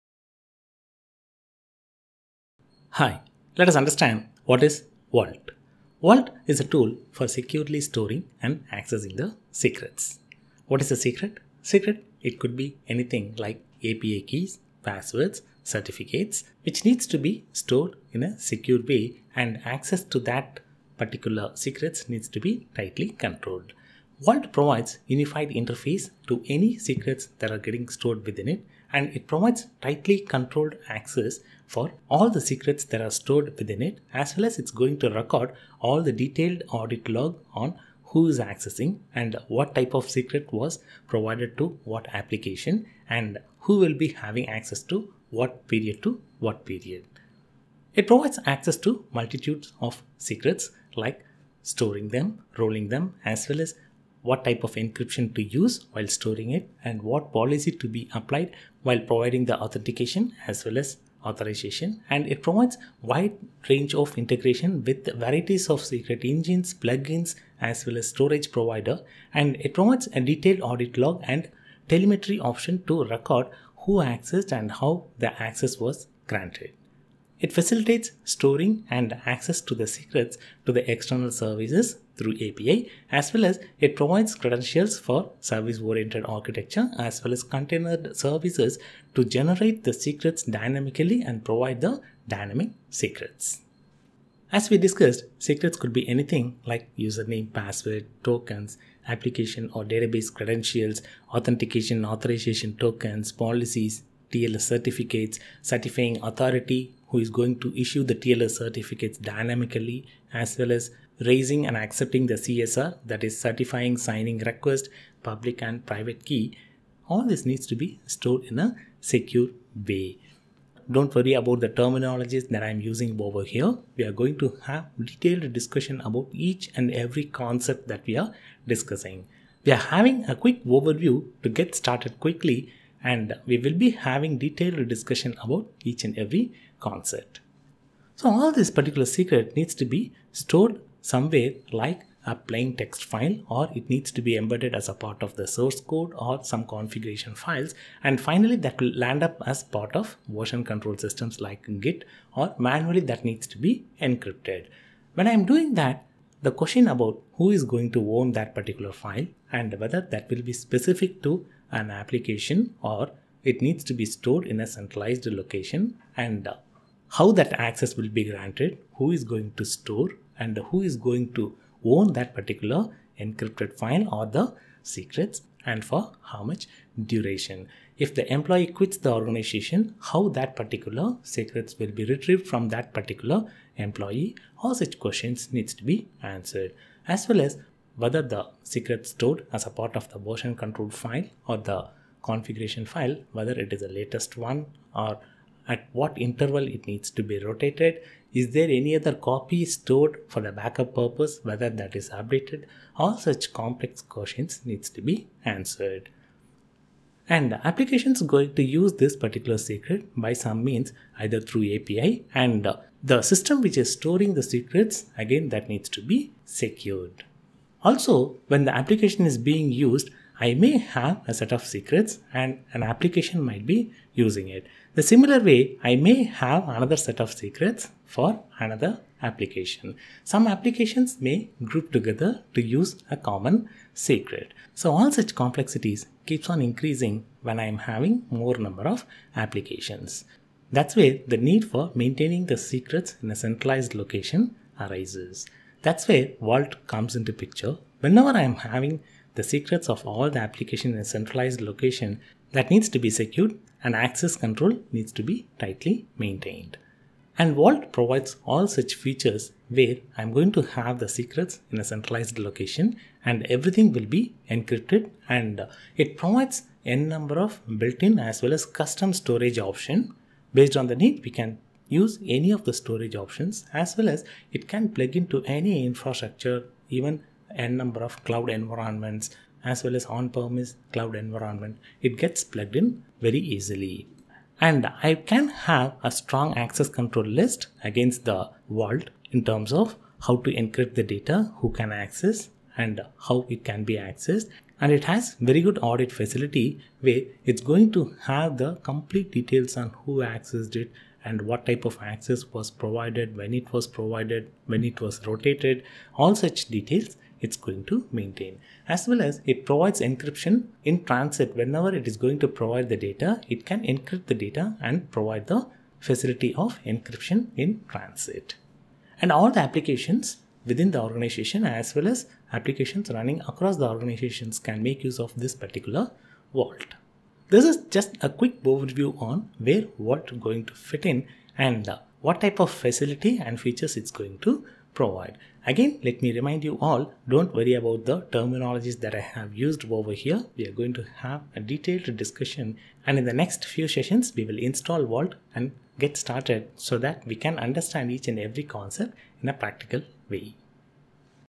HashiCorp Vault Introduction - Video 1
Understand what is Vault and its purpose

Understand the secrets and purpose of storing them in the vault

Beneficiary of vault

Who and How Vault will be used in CI / CD pipeline